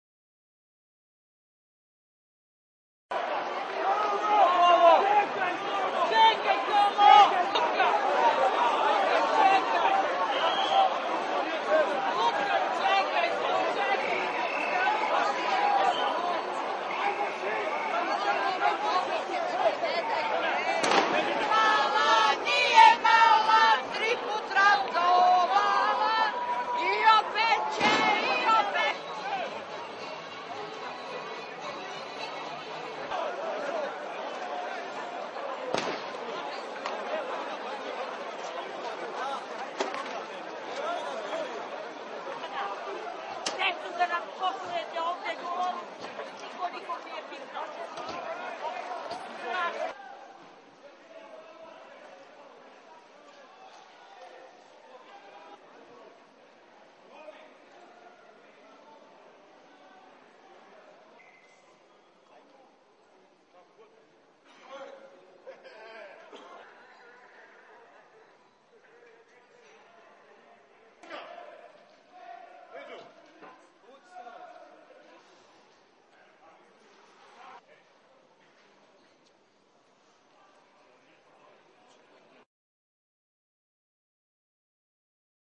Svesrpski sabor u Beogradu 3
29.jul,2008. - Više od 16.000 ljudi okupilo se na protestu Srpske radikalne stranke u Beogradu zbog hapšenja dr.Radovana Karadžića.